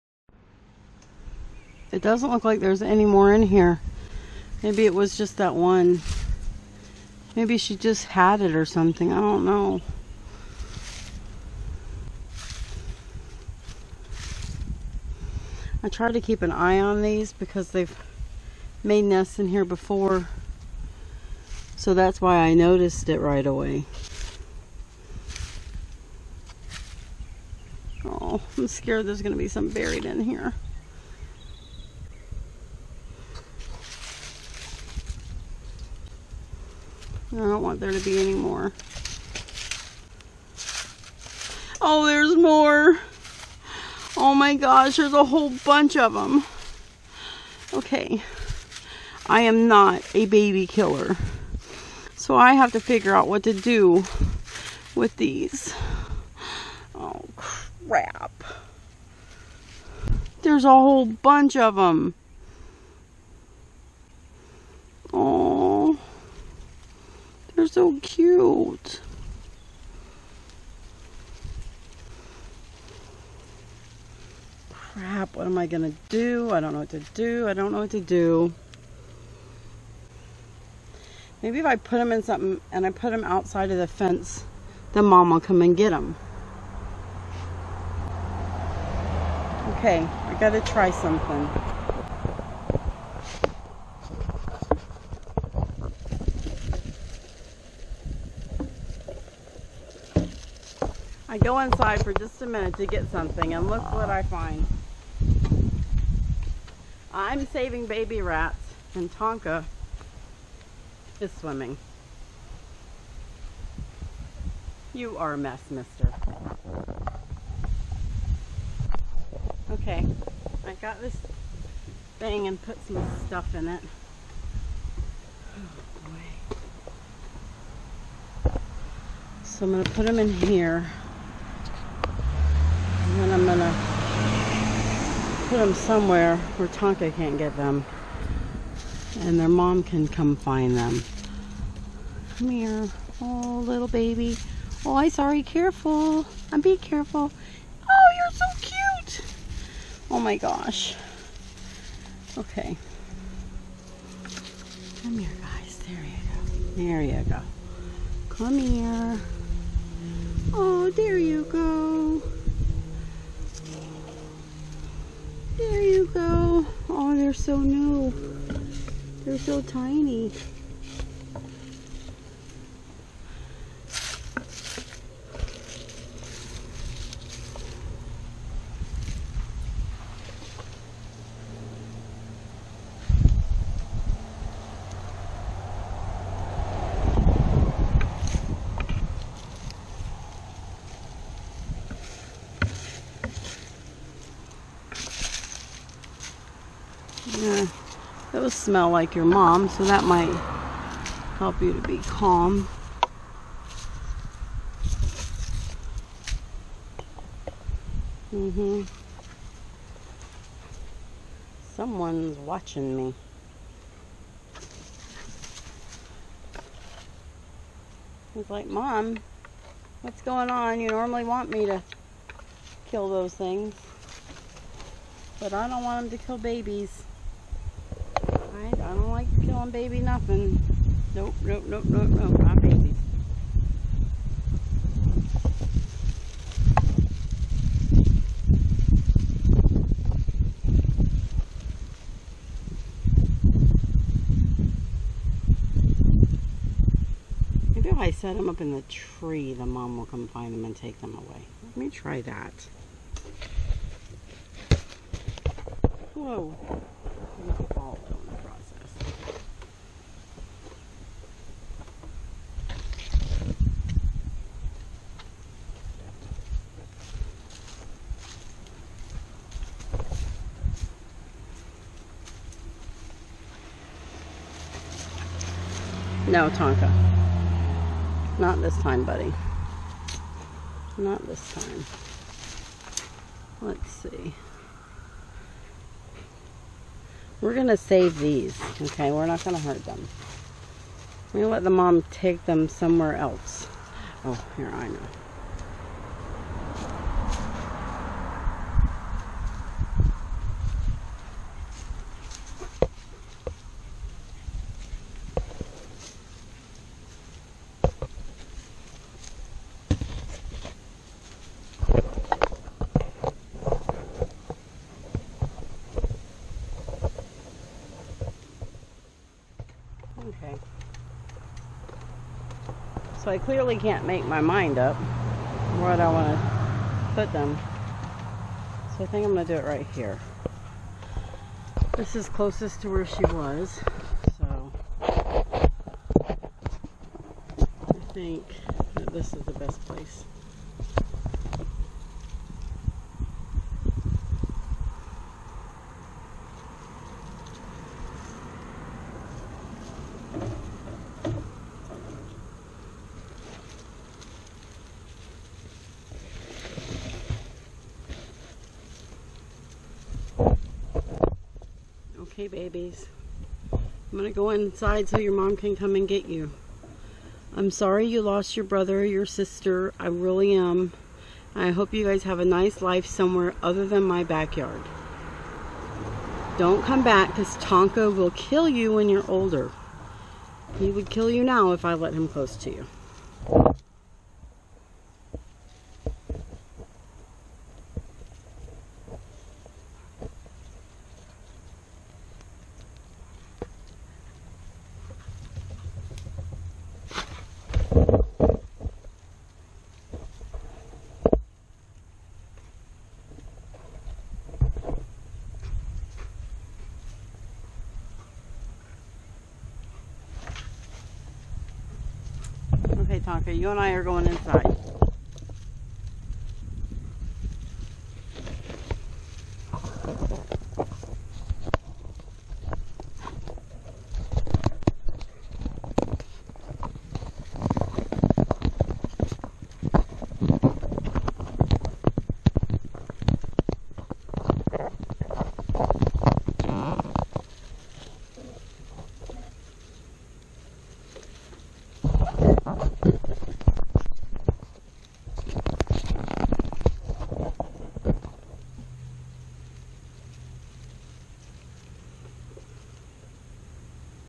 WHAT HAPPENS WHEN A RAT BUILDS A NEST IN AN ALASKAN MALAMUTES YARD Ep 2
I was surprised to see that a rat used our birdhouse to build a nest! There was no nest the night before! This was around 8:00am! That way you will be able to Laugh at all of them :)!